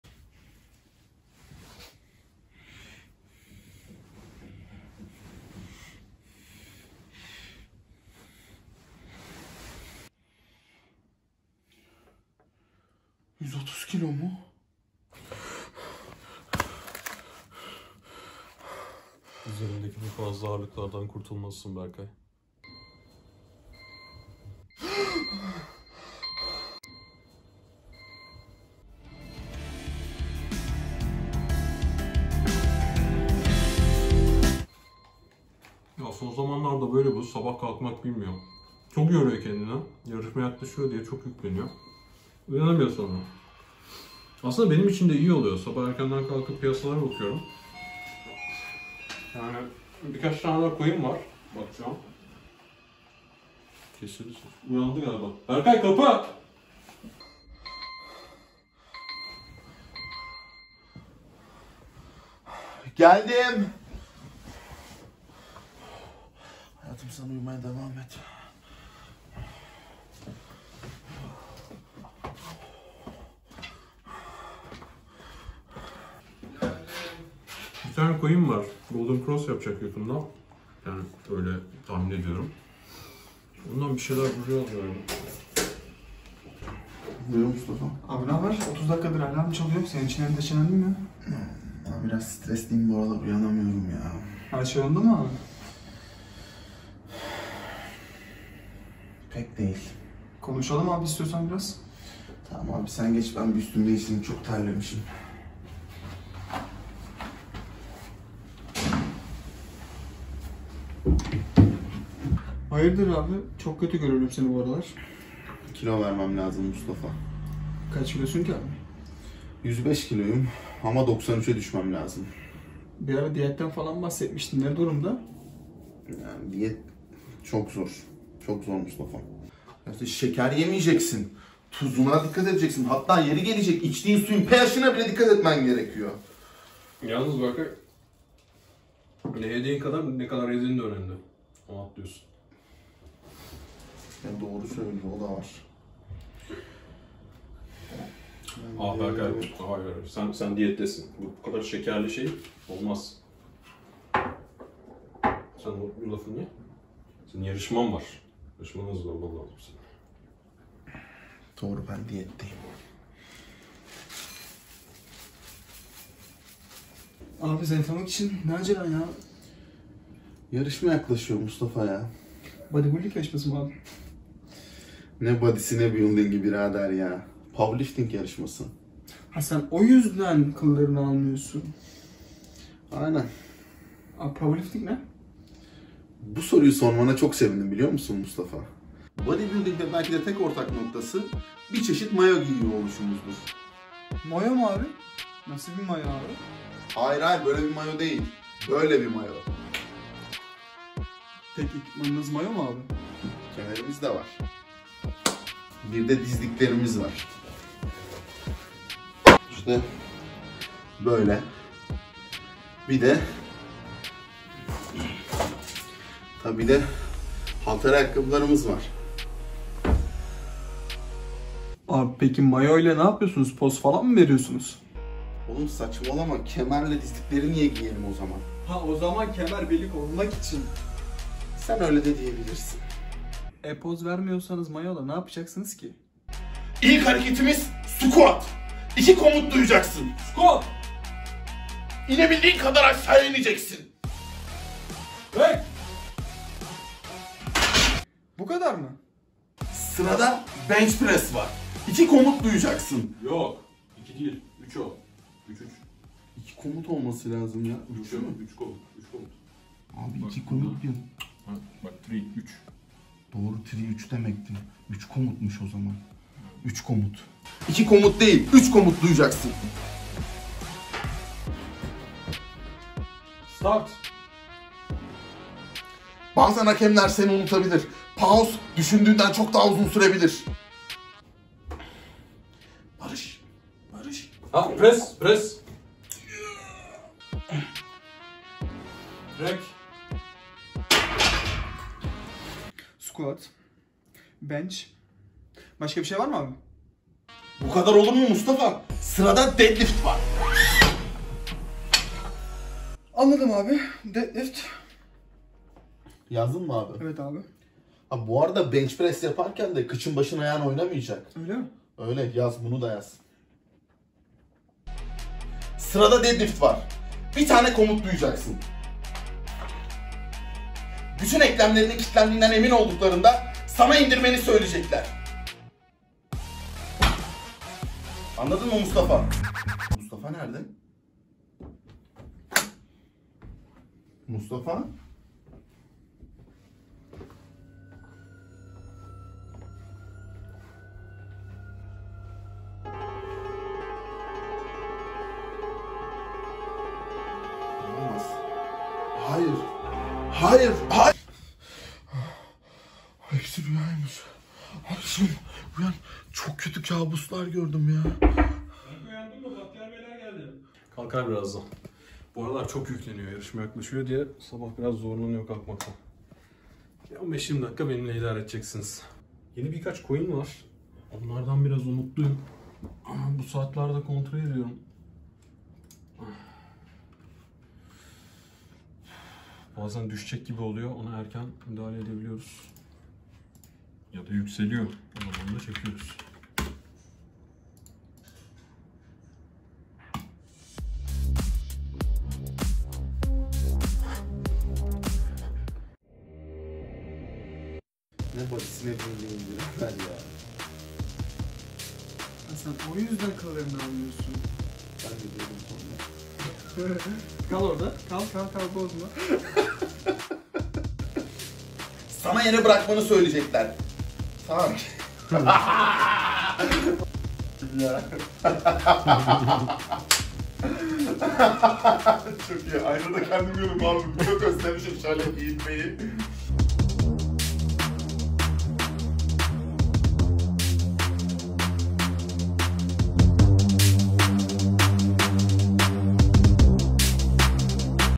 Bekomanci: Bölüm 10 YAKLAŞAN YARIŞMA
IPF Türkiye Powerlifting yarışmasına az kalmıştır. Bekomanci'nin 10 kg vermesi gerekmektedir. Diyet ve antrenmanlar Bekomanci'yi çok strese sokar.

İyi seyirler.